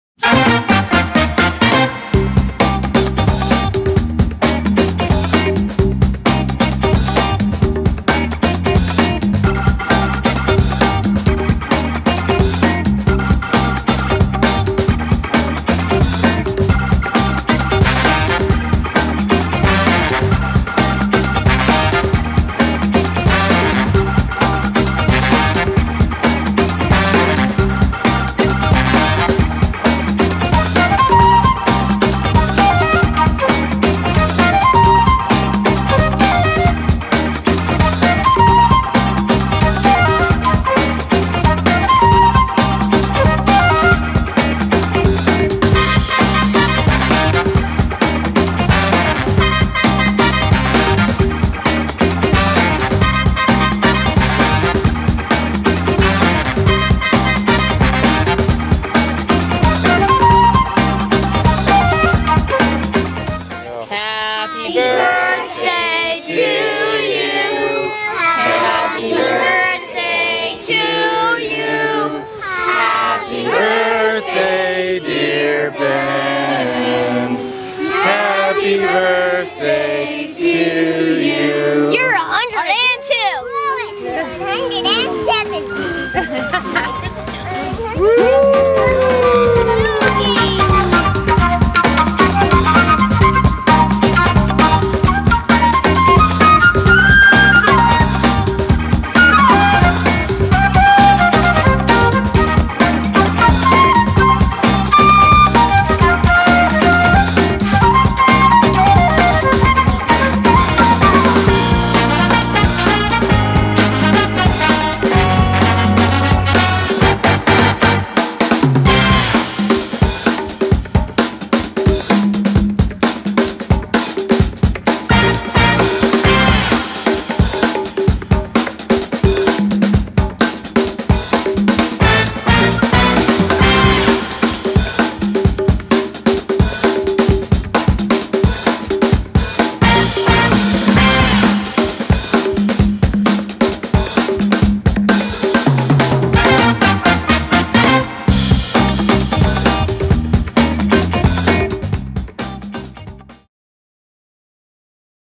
Ben Ben Moore's 5th Birthday Party Bigfork Montana
Benjamin Moore of Woods Bay, Montana is turning 5 years old on October 1st 2010... This is a celebration with his friends and family at Sliters Park in Bigfork Montana